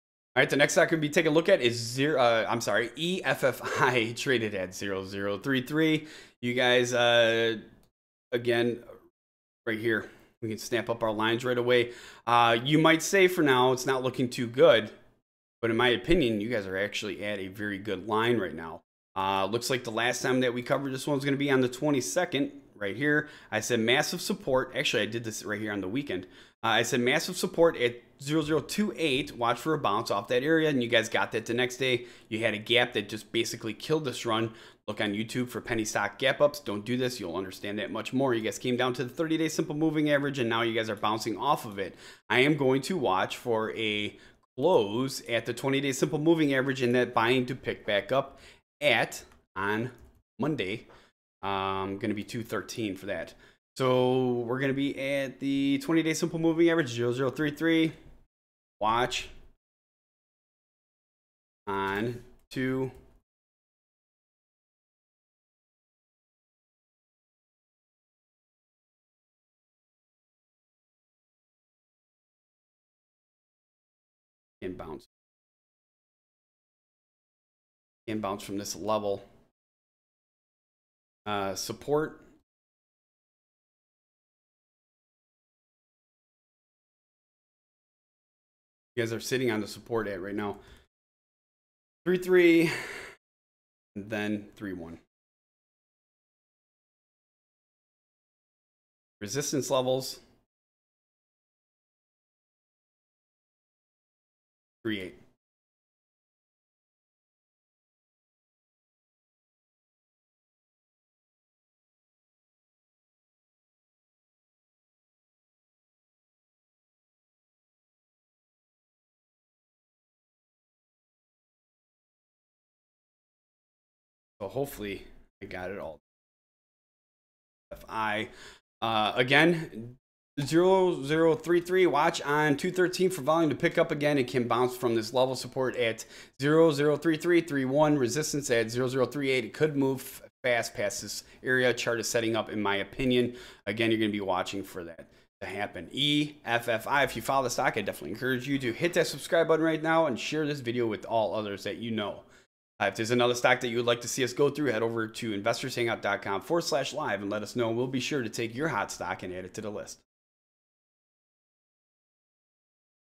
EFFI Stock Live Analysis 02-10-2017
EFFI Stock Analysis (EffTec International Inc.) as of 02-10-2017. $EFFI stock chart analysis along with the current stock price, filings, financials, news and a brief description of the company.